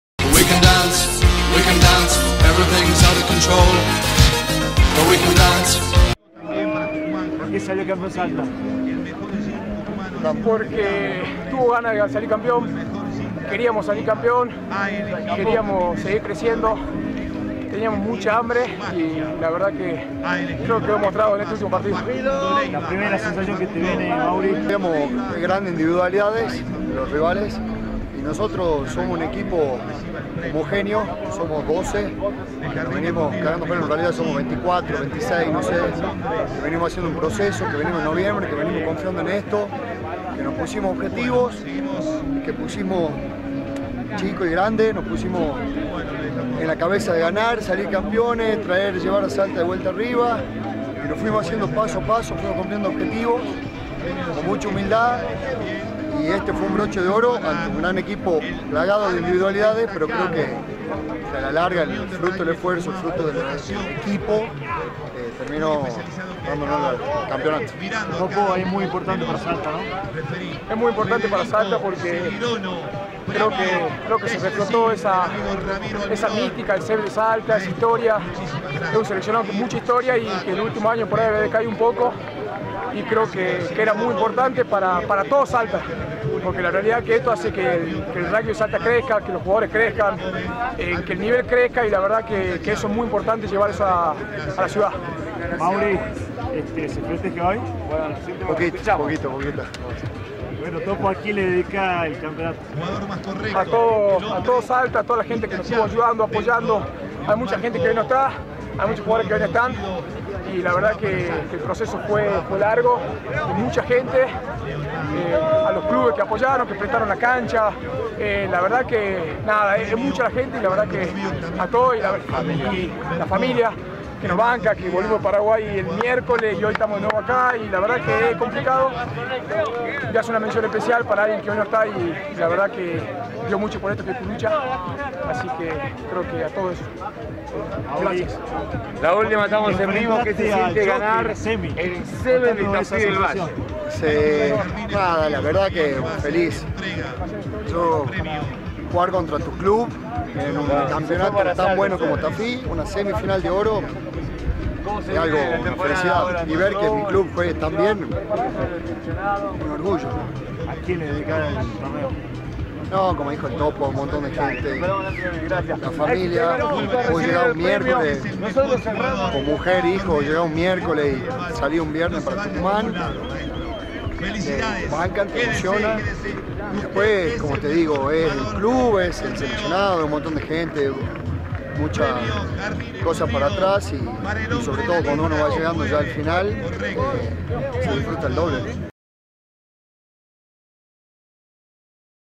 Mauricio Giménez - Federico Salazar
El seleccionado de la Unión de Rugby Salta "NEOGEN", se quedó con la Copa de Oro "La Gaceta" del Seven de Tafí, al vencer en la final a "Pipas-Rugby" por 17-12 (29/01/22).